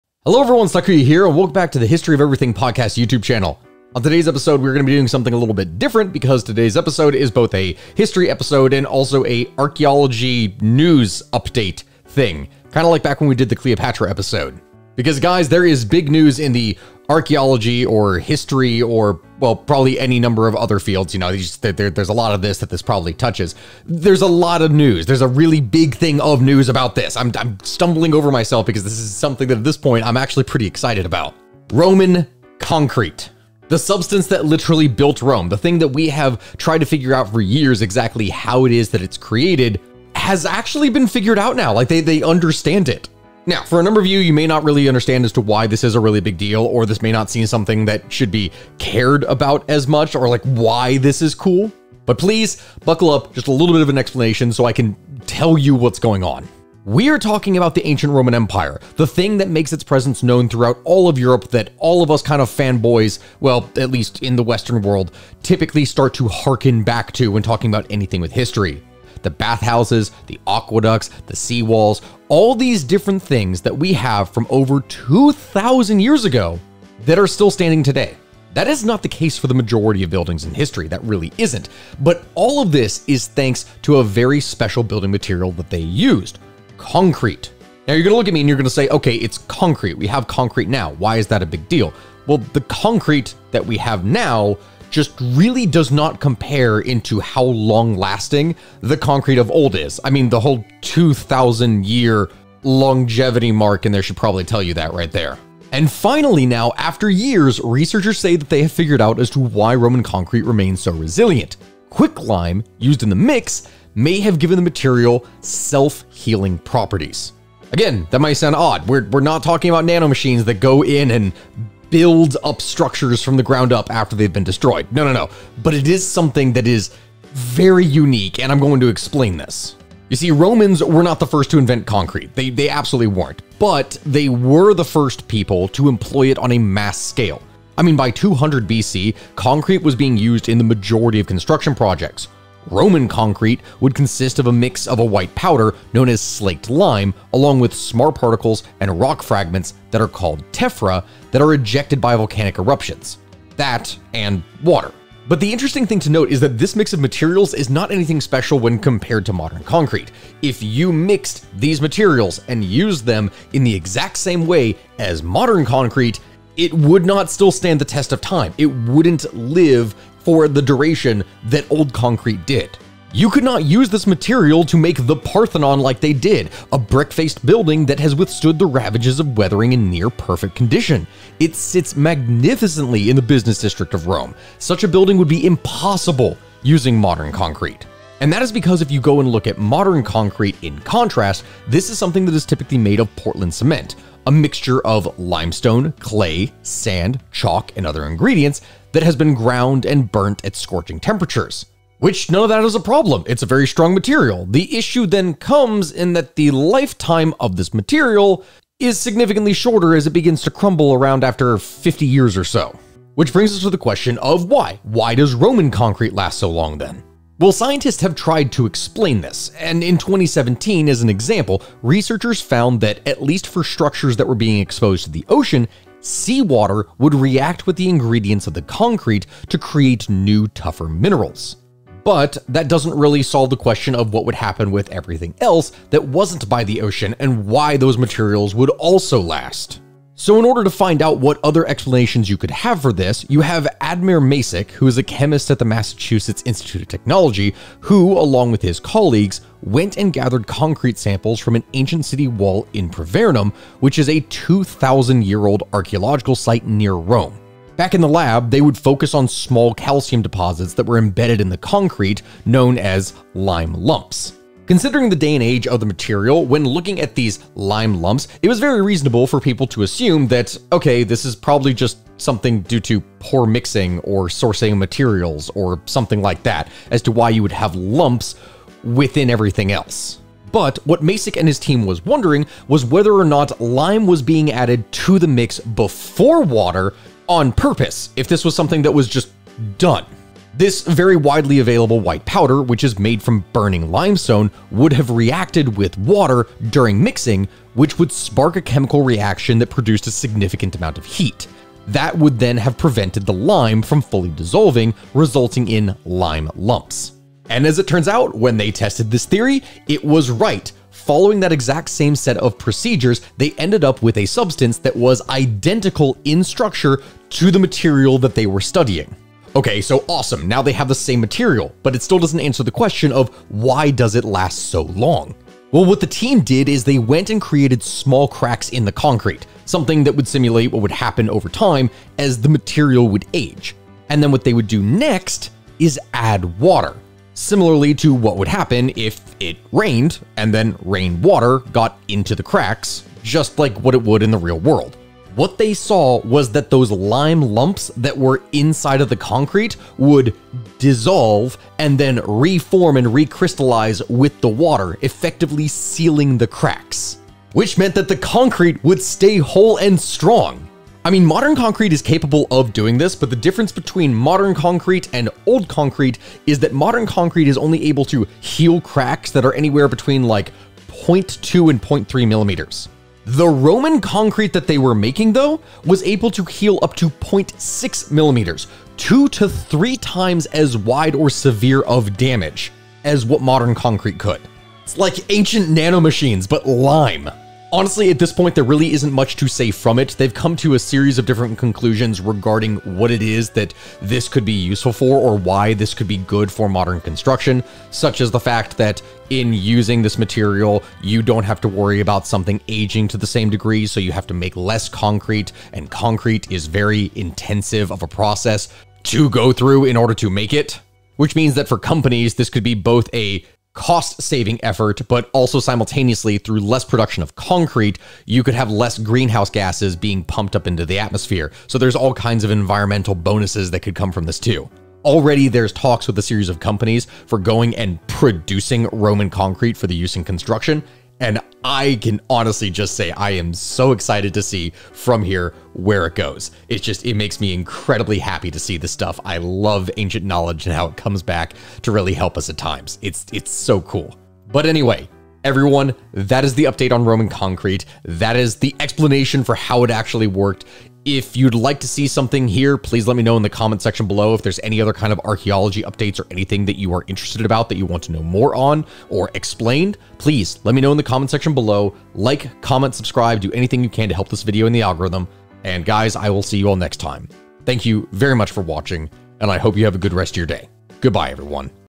We FINALLY Figured Out The Secret of Roman Concrete
Guys this is big news and it makes me really happy. I Cannot wait to see what the future brings with this.